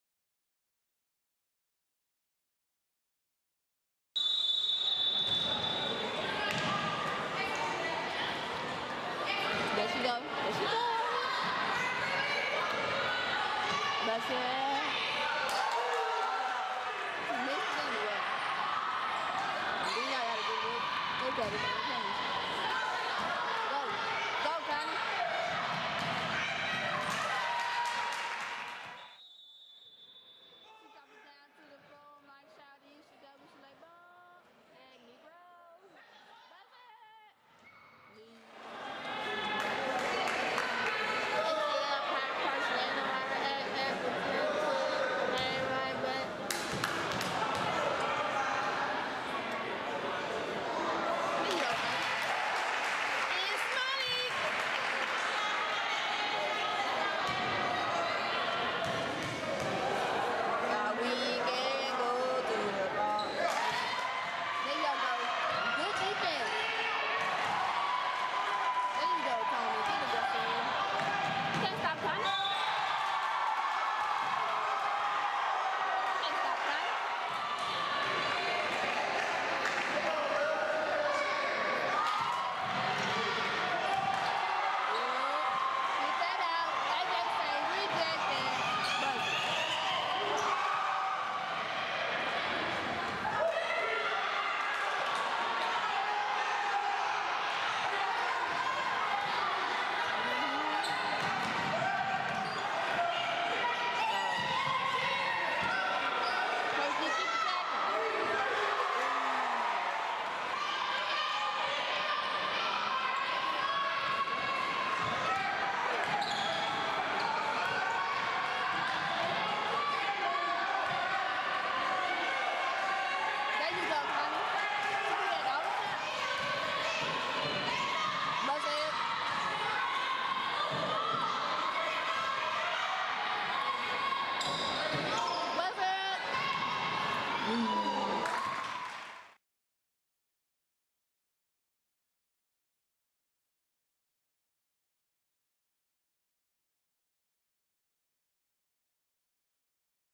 wharton 08 0001
Connie Espinas #23 Junior Yr. STafford high school vs Wharton high school. district game. 07-08.
31 points. VIEW search connie espinas. search connie espinas. search stafford tx lady spartans.